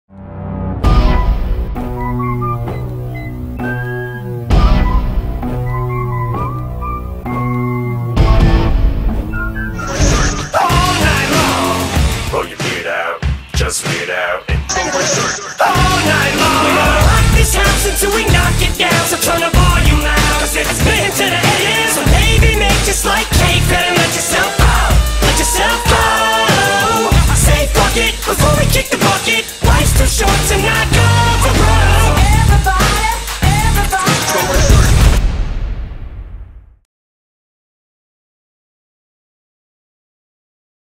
Wakeboard Trailer
Join Connect's Wakeboard Day! If James tried it, so can you! Sign up at the office.